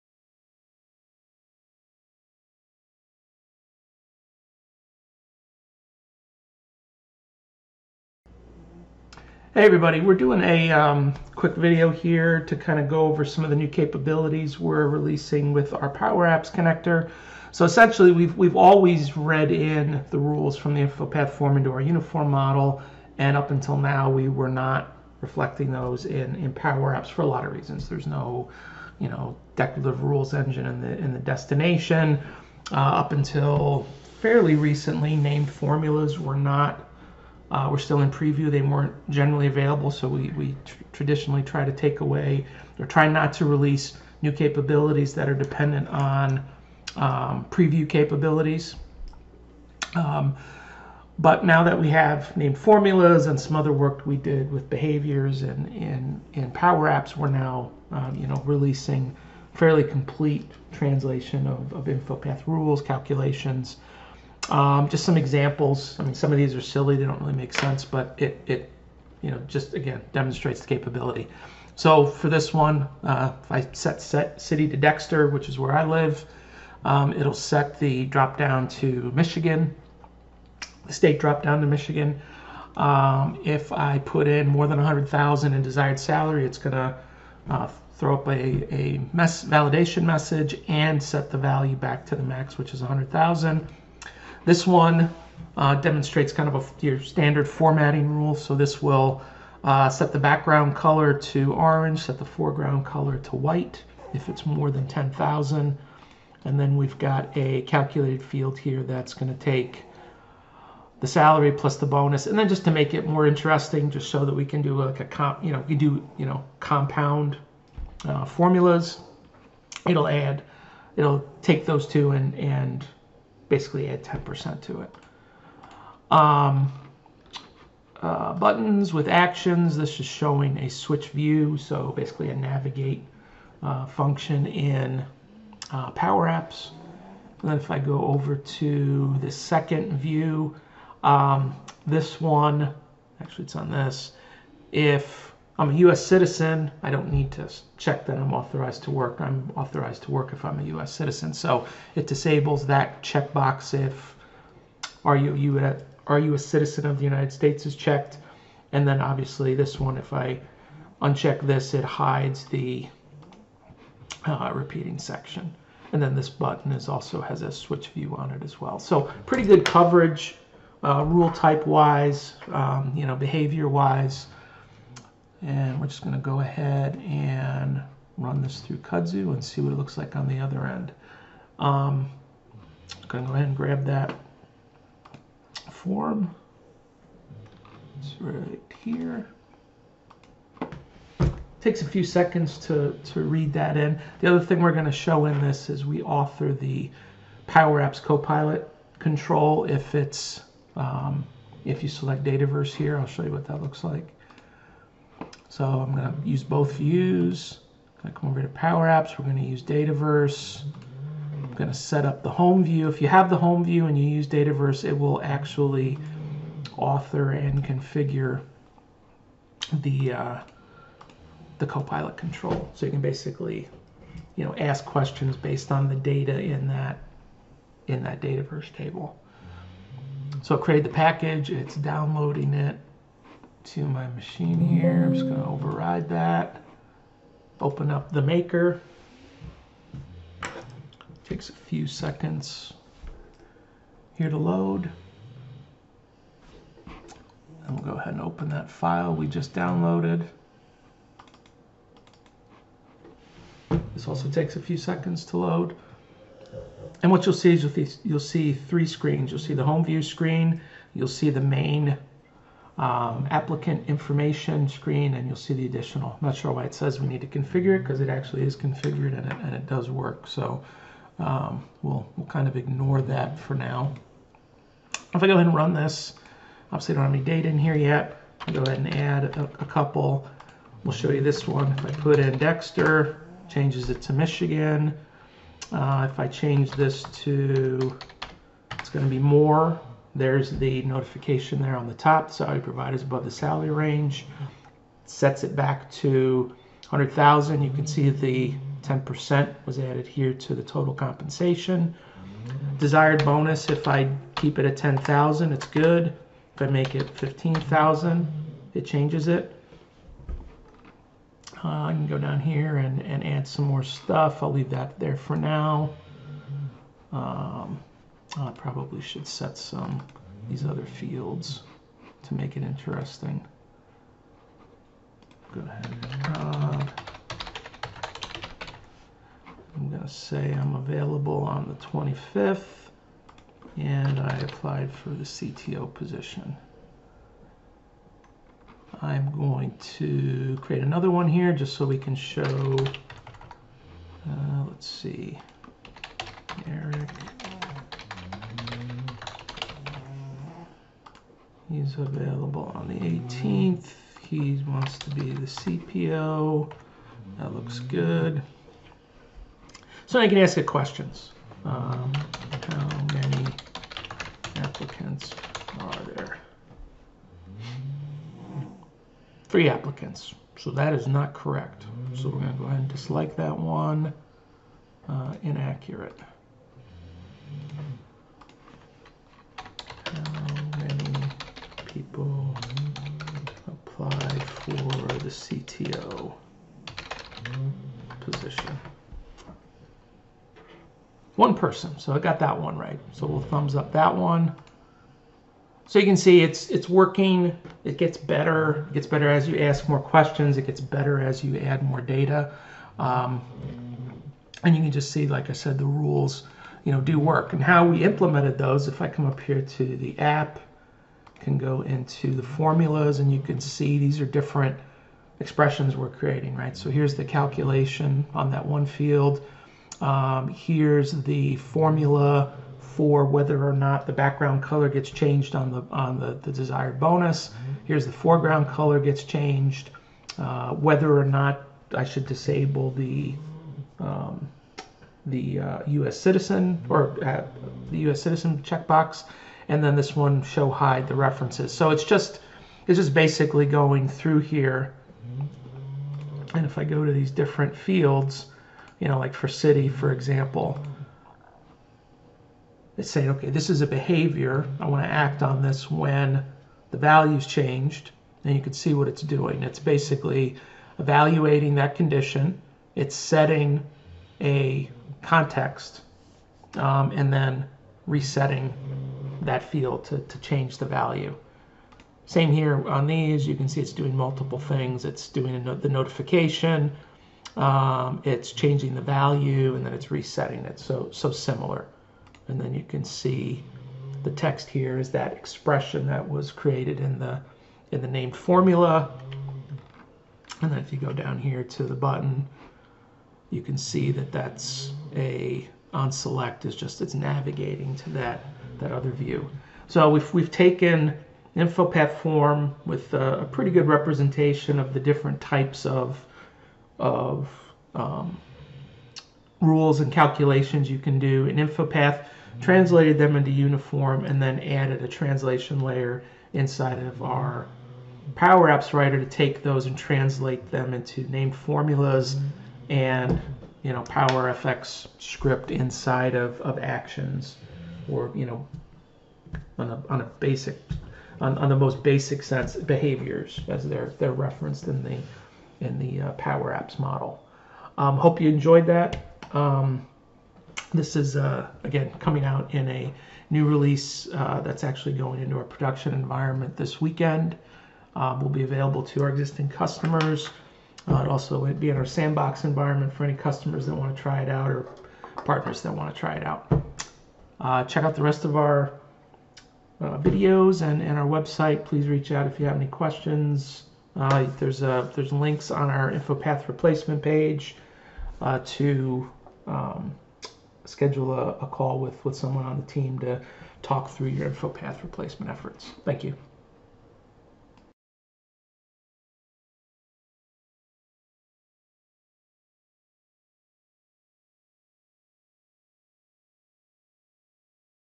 InfoPath to Power Apps
How to leverage Kudzu to convert your InfoPath forms to Power Apps including calculations, rules, actions.  Video also demonstrates how Kudzu can author and configure the Power Apps Copilot control to work with the newly created app.